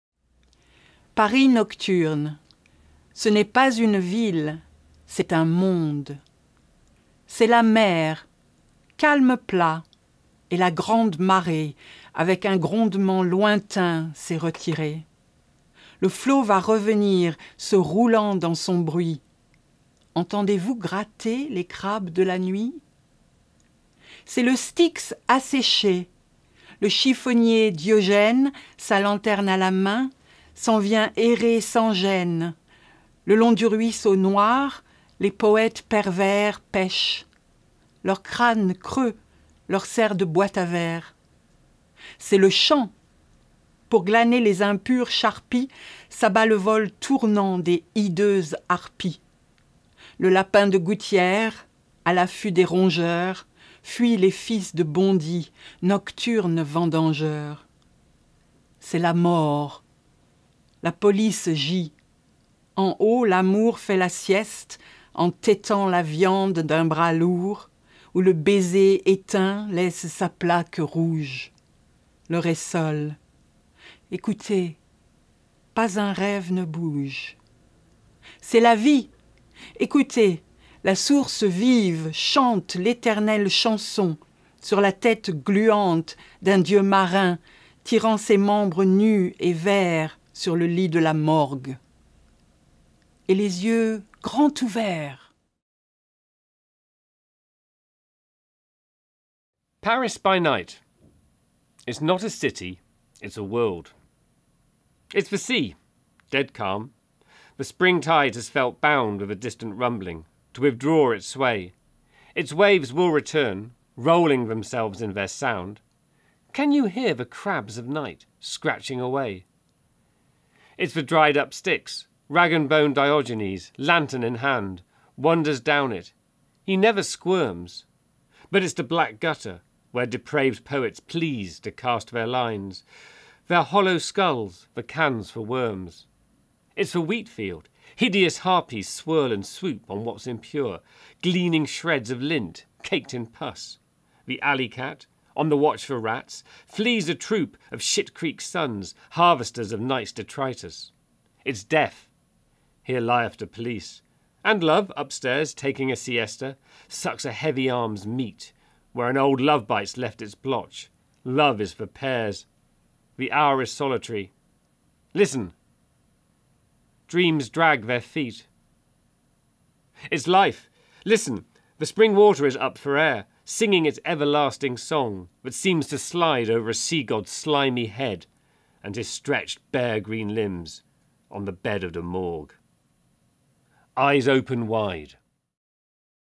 Paris nocturne / Paris by night
A reading in French and English of a poem by 19th century poet Tristan Corbière.

This poem is from the book Oysters, nightingales and cooking pots - selected poetry and prose of Tristan Corbière, translated by Christopher Pilling, edited by Richard Hibbitt and Katherine Lunn-Rockliffe, and published by White Rose University Press.

The volume is free to read online and download where you can also purchase a print copy.